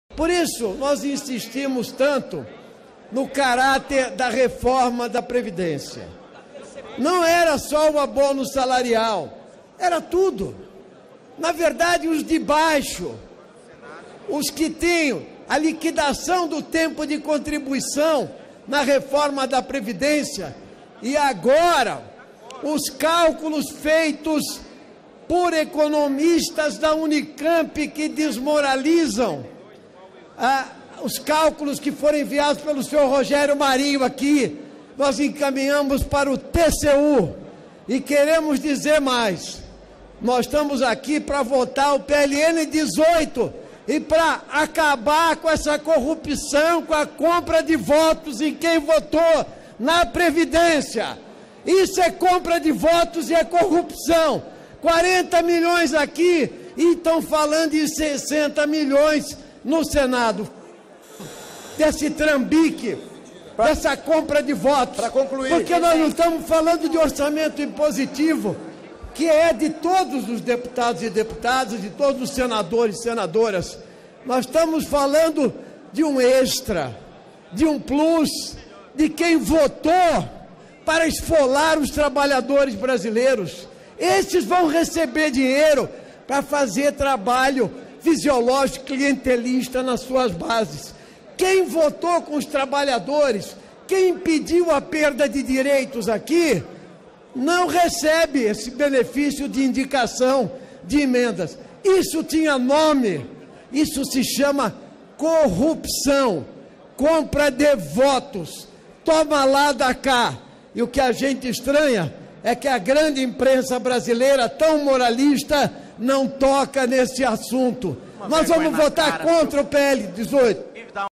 Compra de votos é corrupção
Liberação de emendas orçamentárias dos parlamentares da direita e do Centrão é um escárnio. É o velho troca-troca entre executivo e legislativo.